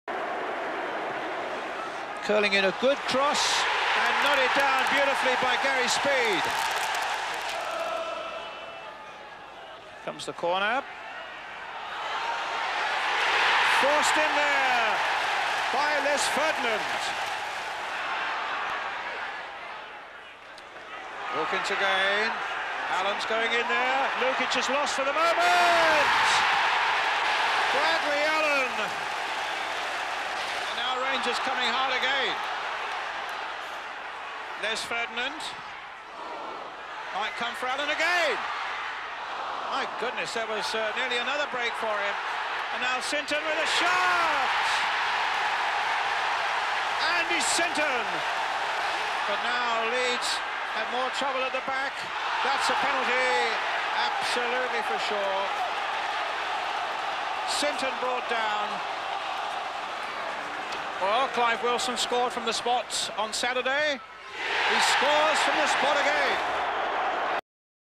Queen´s Park Rangers 4-1 Leeds Utd. 1991-92 First Division
Match played at Loftus Road on March 11th, 1992. For full list of matches on dvd, go